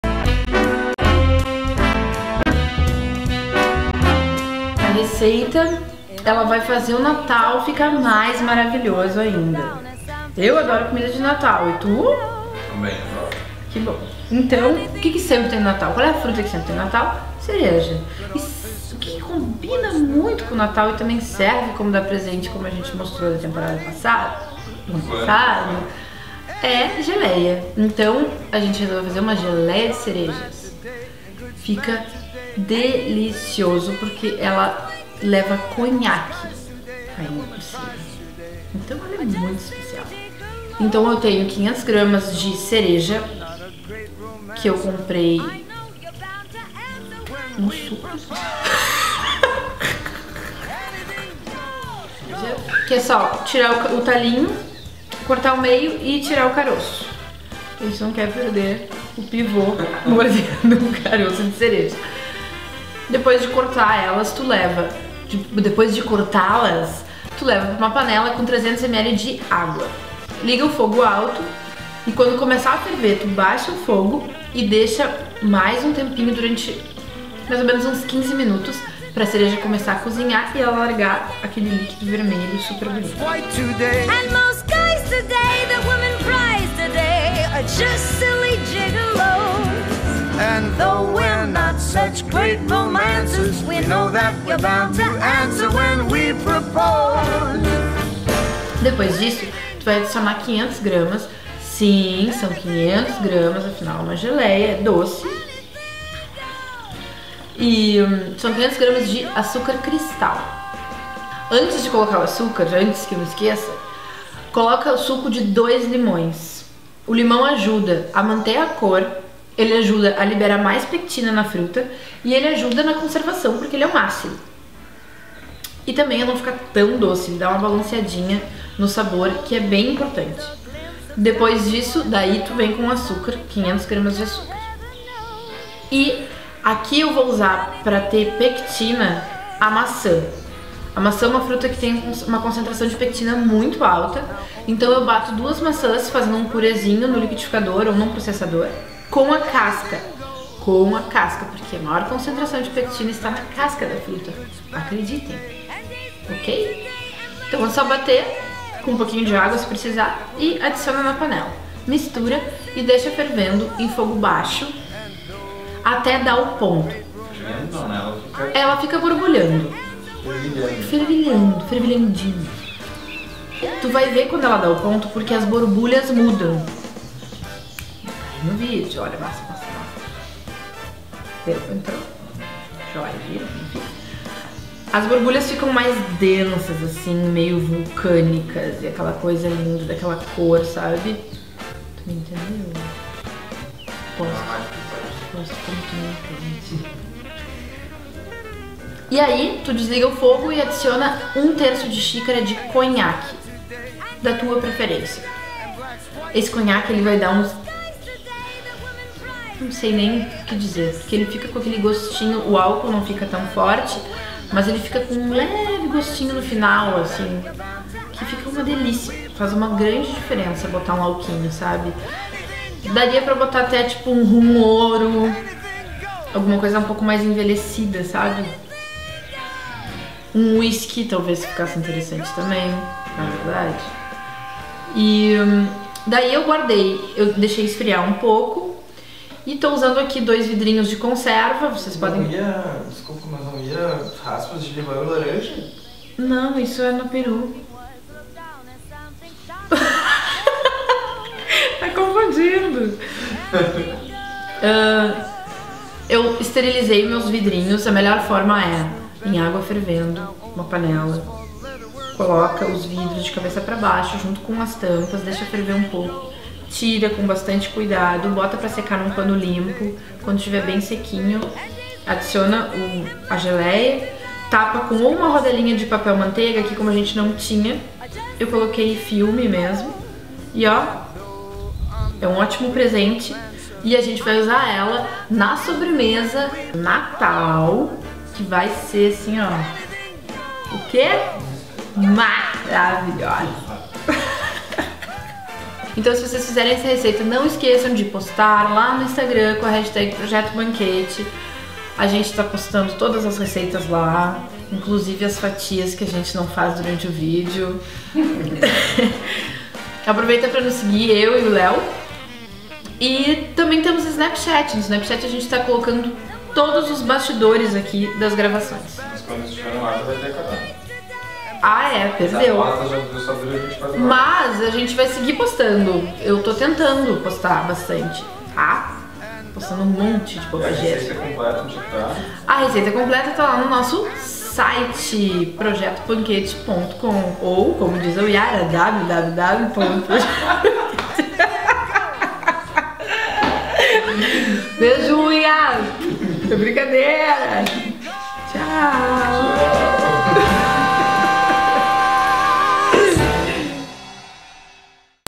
Geleia de Cereja com Conhaque
Pra começar bem o nosso Banquete de Fim de Ano, a Mari ensina uma geleia de cereja para acompanhar as ceias, os jantares e qualquer encontro dessa época.